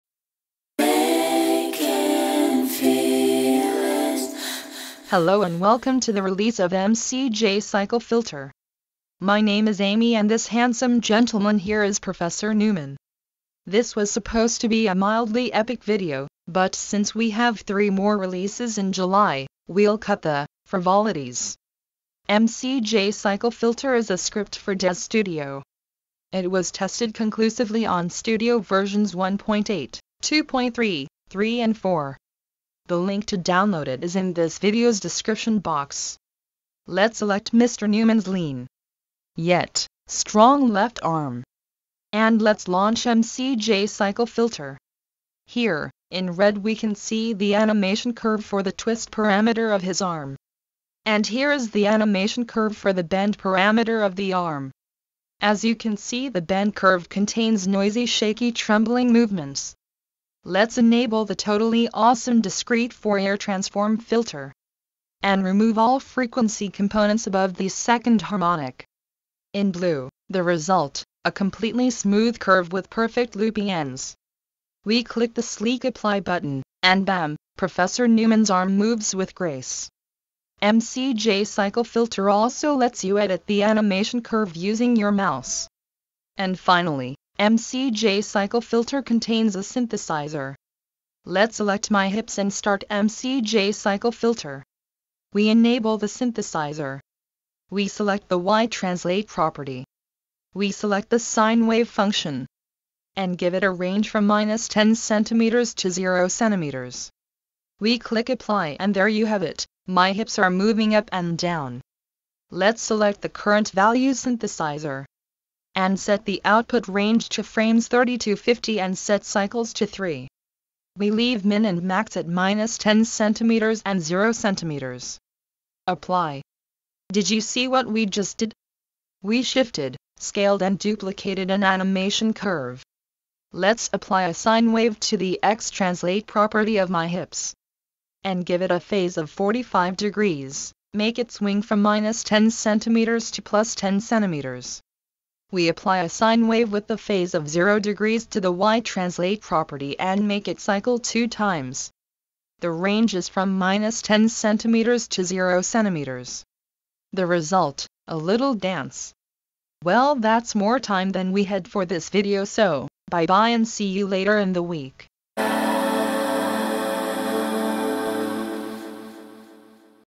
View / Edit / Smoothe / Synthesize Animation curves in Daz|St. : MCJCycleFilter
mcjCycleFilter is a free script for Daz Studio 1.8, 2.3, 3 or 4

it was designed to help you create and perfect short animation loops like dances and walks

you can download it on my well-behaved  google site :)